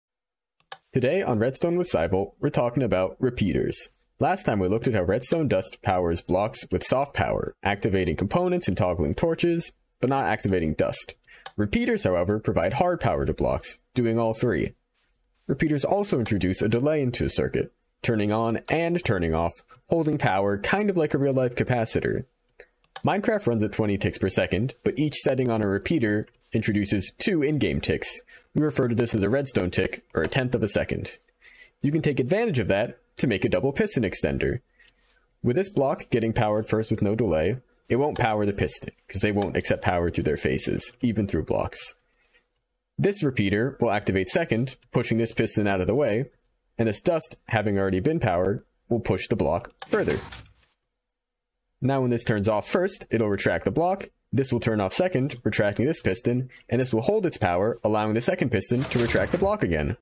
REDSTONE REPEATERS basics and DOUBLE PISTON EXTENDER | Redstone with PsiVolt
@PsiVolt Today we're looking into some of the basics on how repeaters power circuits and how to utilize the delay they introduce with a tutorial on a double piston extender!

(And just about anywhere else you find music @PsiVolt)